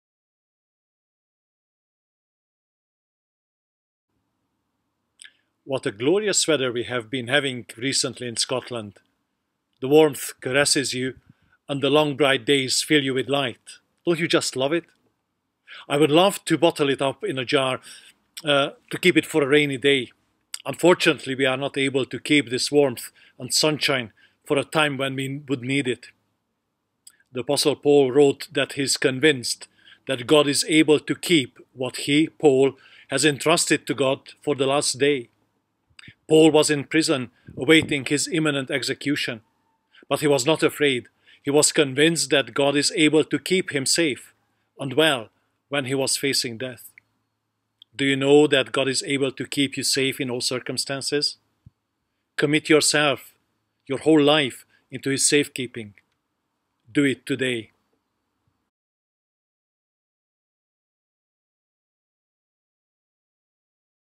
60 Second Sermon God is Able to Keep
60 second long (or short) sermon about God being able to keep for ever all that you entrust to Him for safekeeping (2 Timothy 1:12)